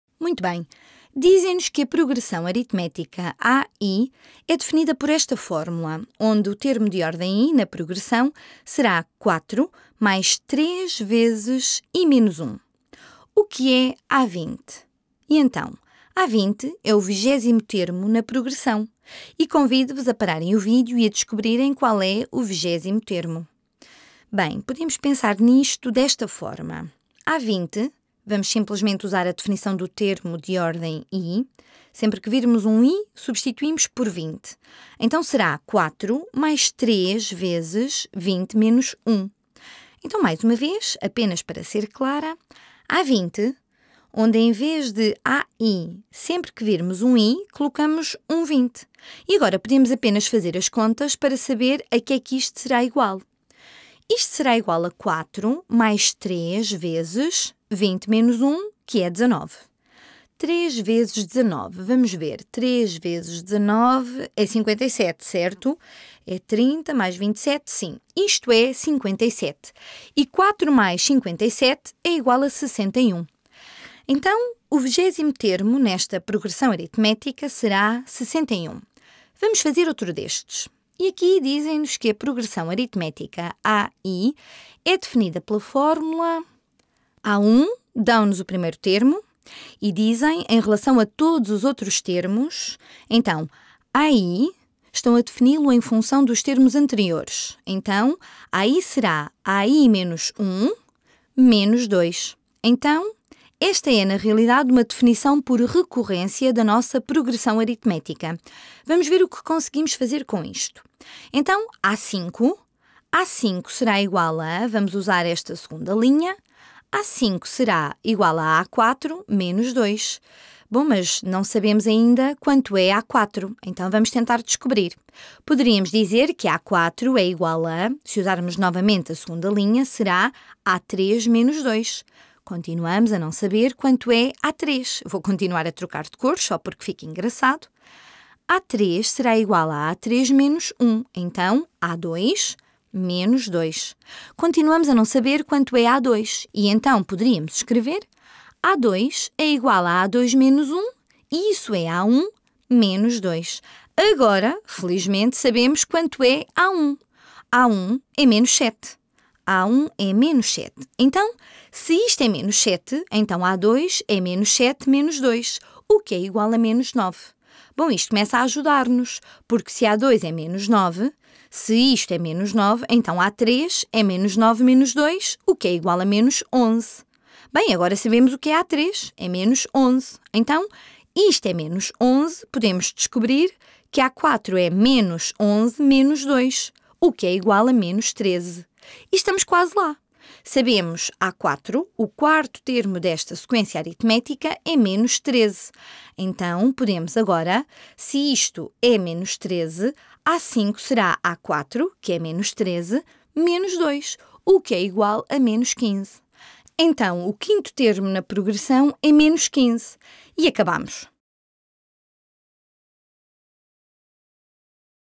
Utilizar fórmulas de sequências aritméticas - Khan Academy em português (7º ano)
Vídeo original: Using arithmetic sequences formulas